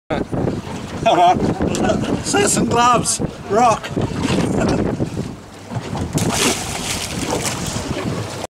Assassin All Day Gloves Rock
Got some of these new Assassin all day jigging and top water gloves, awesomeness, you should get a pair. Great quality and very comfortable.